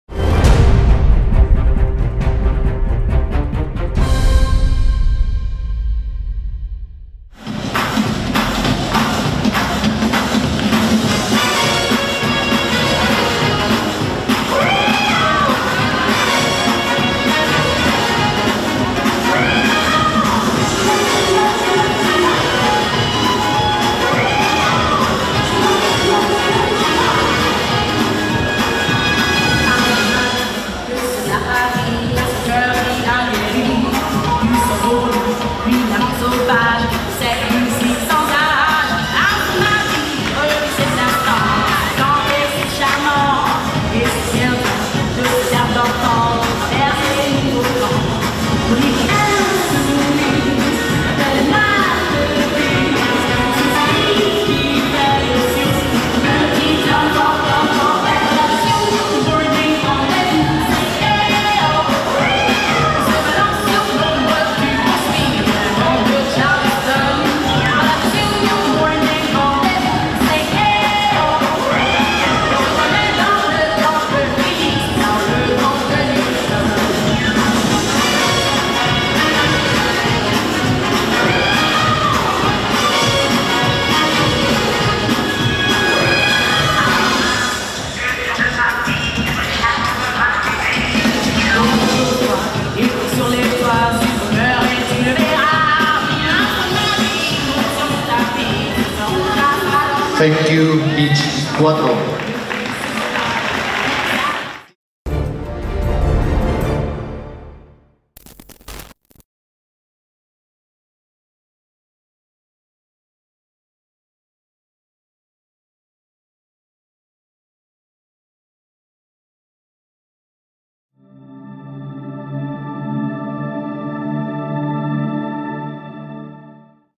Kecskemeti - Fellinghauerova, CZE | 2016 World Under 21 Std Platja d'Aro | R1 Q | DanceSport Total
Kecskemeti - Fellinghauerova, CZE | 2016 World Under 21 Standard Platja d'Aro | R1 Q

Daniel Kecskemeti - Barbora Fellinghauerova, CZE, dance the Quickstep in round 1 of the 2016 World Under 21 Standard on 29 october in Platja d'Aro, ESP.